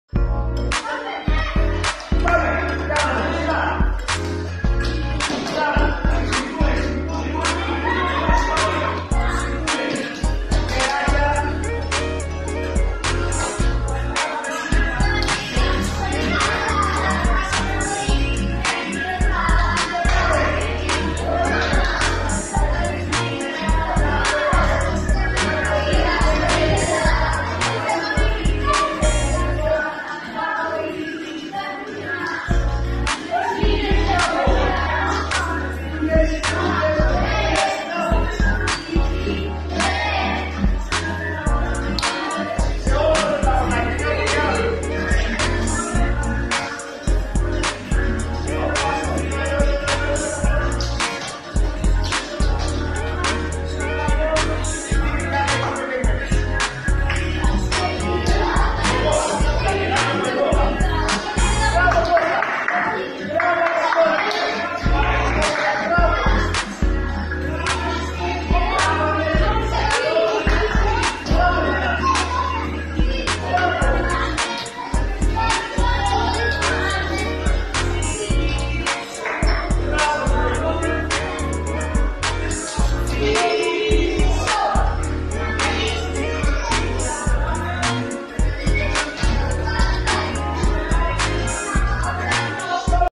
AcademicaFc14 Training Day
Προπόνηση Κ7 Κ8 τμήματα σε κλειστή αίθουσα λόγο βροχής